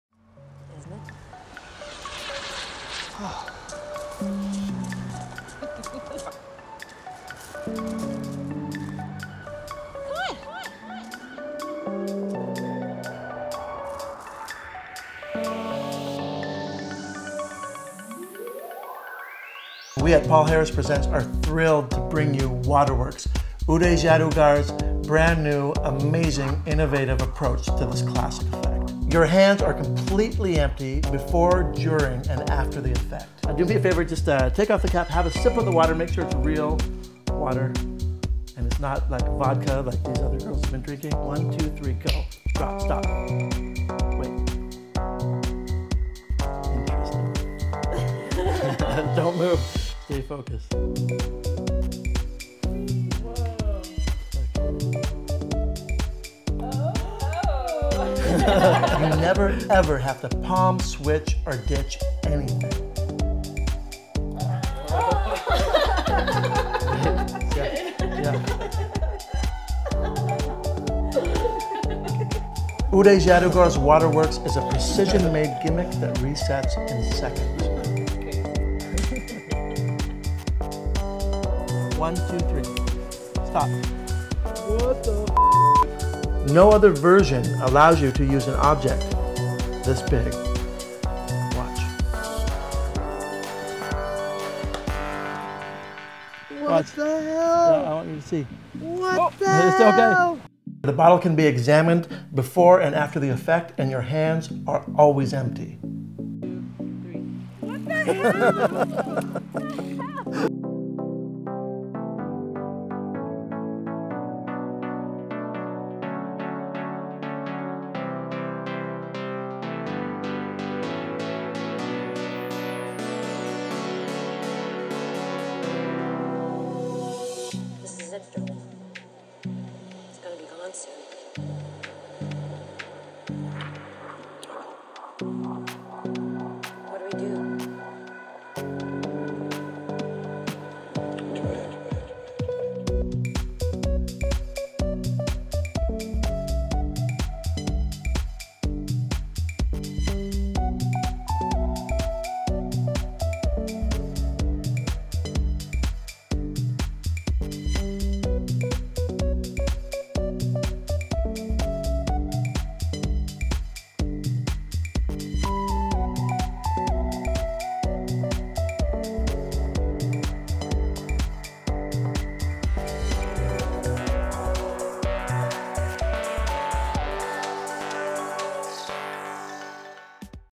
Paul Harris Presents Water Works (DVD and Gimmicks) by Uday Jadugar
An astonishing new method for the easiest, most powerful anti-gravity water effect...Ever. The good news is; You never have to hide anything in your hands! Plus there are no chemicals or sticky stuff involved. Your spectator can even take a sip of water from the bottle during the performance! Your hands are 100% clean... At ALL times! Basic Effect: Your always-empty hands take a normal examined clear plastic bottle of water and turn it mouth down. There should be a messy flood of water...but instead, there are just a few drips...and then...nothing! The water stays impossibly suspended inside the upside-down open bottle. The cap and bottle are inspected and there is nothing to find!! No Ditching! No Palming! No Stealing! And even cooler than that...you then push a FULL SIZE pencil or pen up into the open bottle mouth...completely in the water! No other version allows you to put such a huge object into the BOTTLE! You then turn the bottle mouth end up...all is back to normal. Your spectators can completely examine EVERYTHING. No Palming. Your fingers never ever have to load or steal anything! WaterWorks' exciting new gimmick makes this 100% self-contained. BONUS: A Great Free Optional Presentation: WATERBALL. Water Ball: A weird science 'experiment' where a spectator drops a special blue ball into the open bottle of water. The ball slowly sinks down through the water. You turn the bottle mouth down...the blue ball slowly sinks down again...and finally drops out of the bottle...somehow leaving the water suspended! This cool visual is also a brilliant convincer that nothing is secretly blocking the water. So exactly how easy is Waterworks' The normal actions you see in the demo is all there is to do. Period. The ingenious new gimmick does all the secret stuff for you! * No Palming. Your hands and fingers are always empty! * Completely Self-Contained. Use the same gimmick over and over. Resets in seconds. Perform completely surrounded. Spectator can take a sip from the bottle. Ingenious new gimmick allows everything to be examined. A normal size pen or pencil can be pushed into the suspended water.Uday Jadugar's WaterWorks comes complete with precision-engineered gimmick, special blue WaterBall, custom keychain carrying case and instructional DVD. We even include a normal water bottle...so you can get your Waterworks working right away!